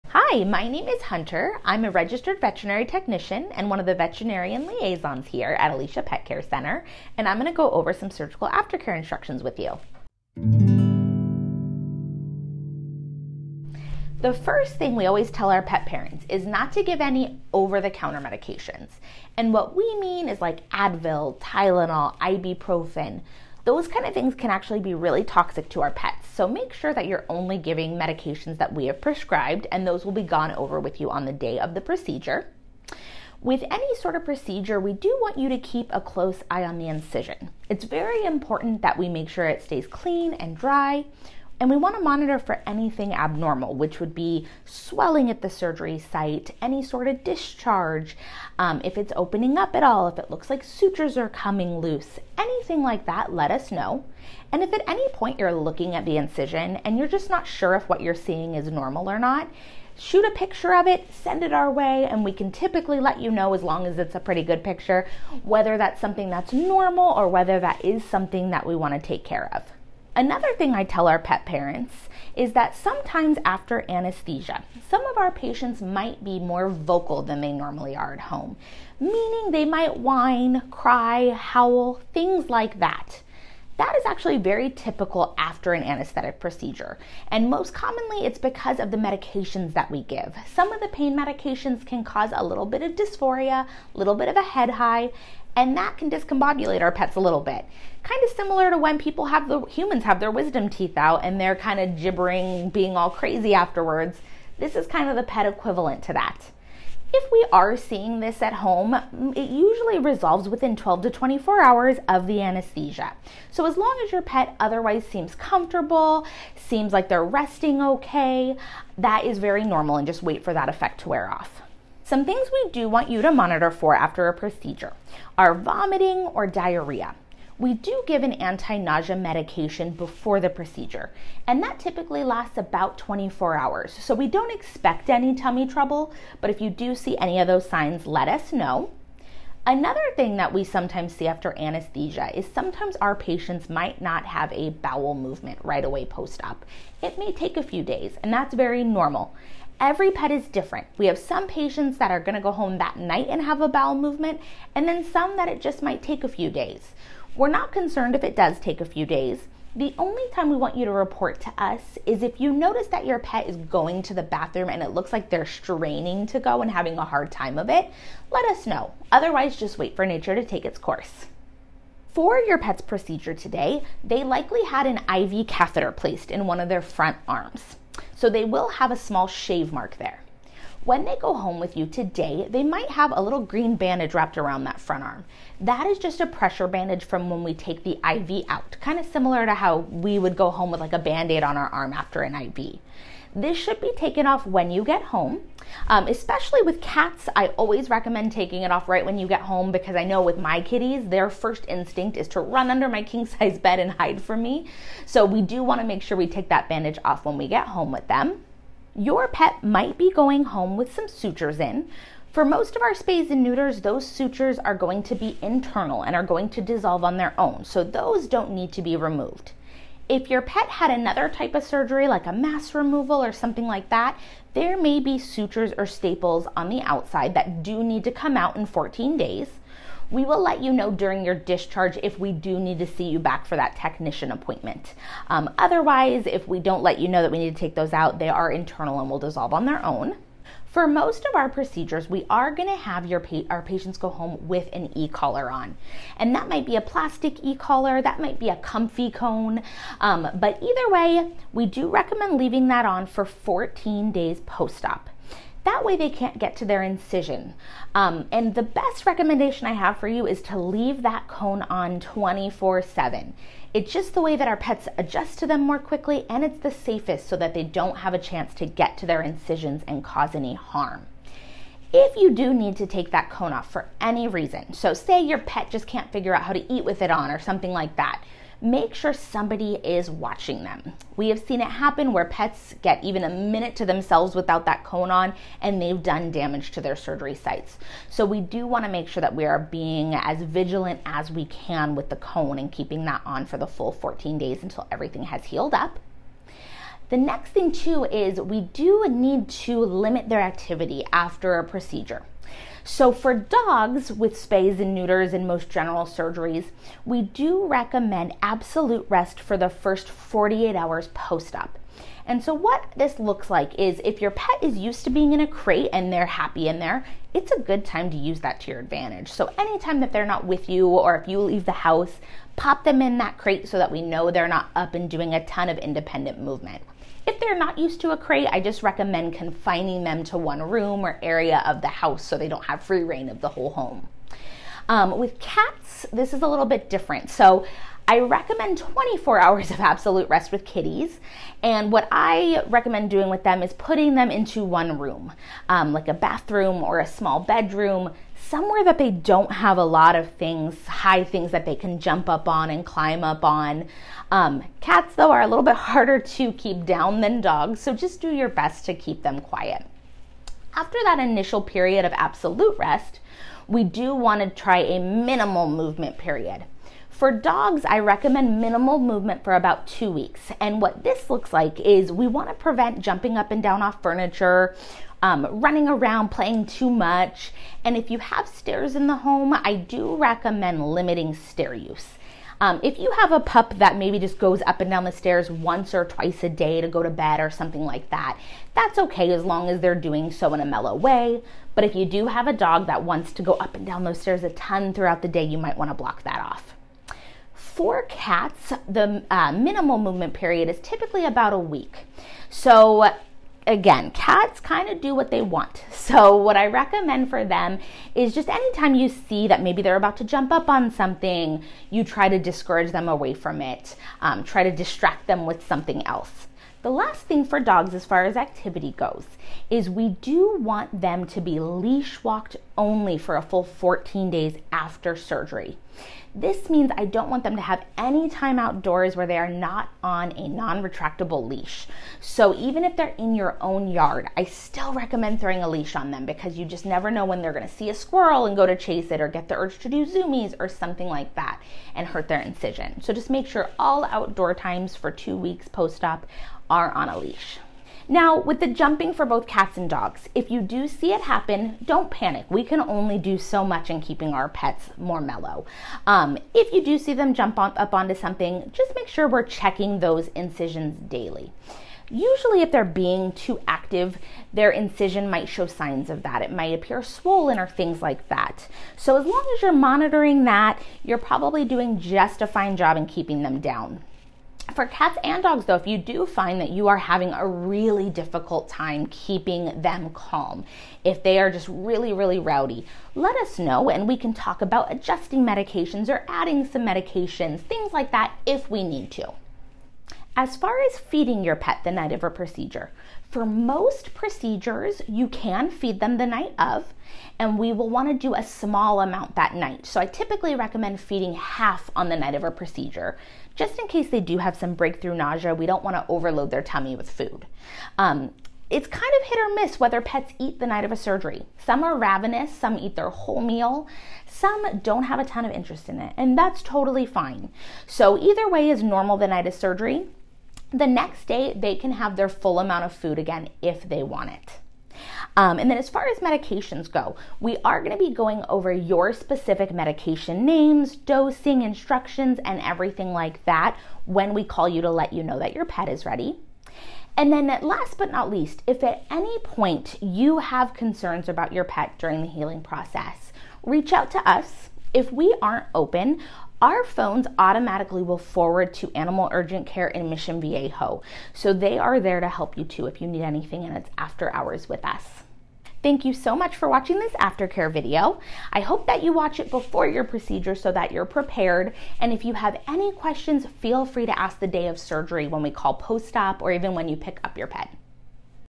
Surgery After Care Instructions
In this video, R.V.T. Hunter goes over surgery after care instructions that we give for pet parents who just had a pet go through surgery.

We recommend our pet parents watch this prior to picking up their baby, so they can bring any and all questions to us!

Chapters:
0:00 Introduction
0:15 Don't Give Over The Counter Meds
0:36 Monitor the Surgery Site
1:12 Expect Patients To Be Vocal
2:05 Monitor for Vomiting, Diarrhea, and Straining
3:15 Remove Bandage Wrap
3:40 If Applicable, Return in 14 Days to Remove Staples/Sutures
4:05 Leave Cone on for 14 Days
5:00 Limit Activity Level
8:10 What to Expect with Feeding
8:47 We Will Give Medication Instructions
9:02 Call us With any Questions or Concerns!

If you prefer to read these instructions, visit this for the video transcription! (and related memes!)